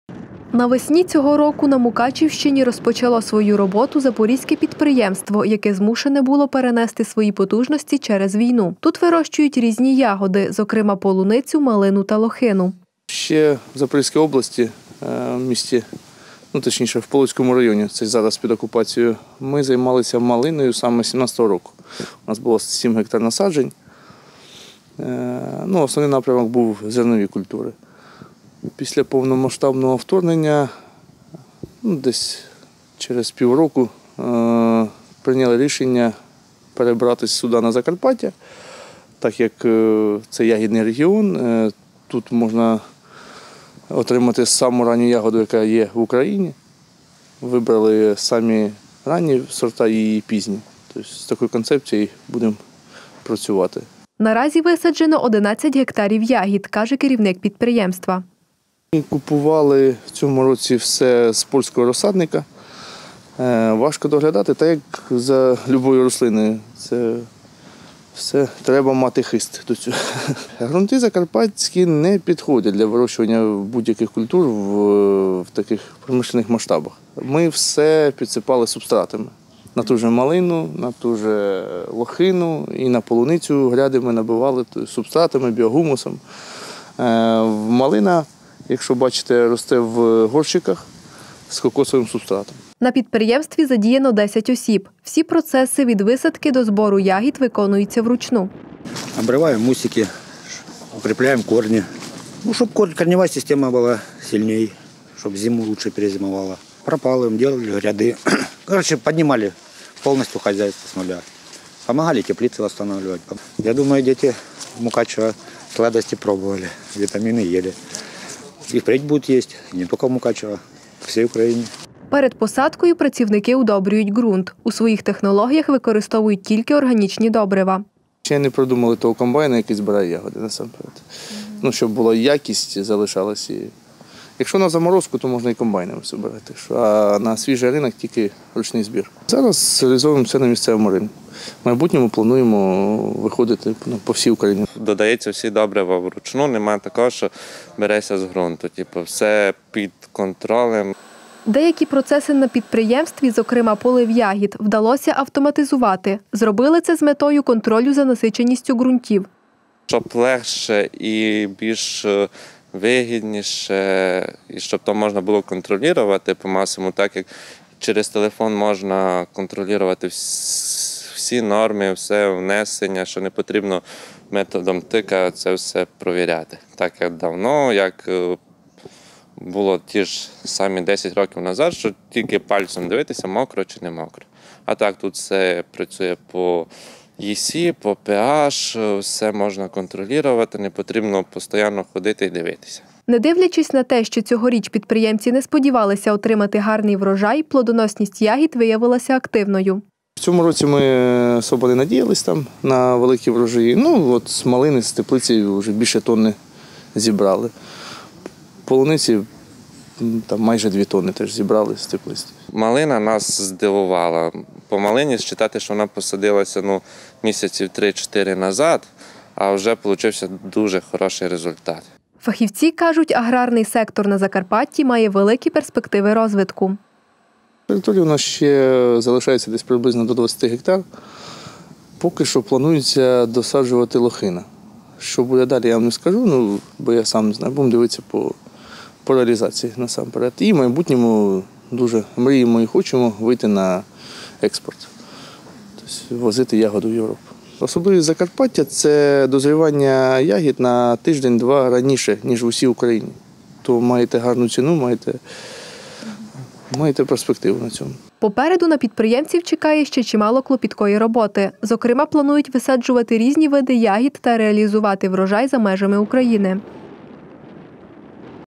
Успішно релокувалися із Запорізької області
На весні цього року на Мукачівщині розпочало свою роботу запорізьке підприємство, яке змушене було перенести свої потужності через війну. Тут вирощують різні ягоди, зокрема полуницю, малину та лохину. Наразі висаджено 11 гектарів ягід, каже керівник підприємства. На підприємстві задіяно 10 осіб. Всі процеси від висадки до збору виконуються вручну. Перед посадкою працівники удобрюють грунт. У своїх технологіях використовують тільки органічні добрива. Деякі процеси на підприємстві зокрема полив ягід, вдалося автоматизувати, зробили це з метою контролю за насиченістю грунтів. Не дивлячись на те, що цьогоріч підприємці не сподівалися отримати гарний врожай, плодоносність ягід виявилася активною. Фахівці кажуть, аграрний сектор на Закарпатті має великі перспективи розвитку. Попереду на підприємців чекає ще чимало клопіткої роботи, зокрема планують висаджувати різні види ягід та реалізувати врожай за межами України.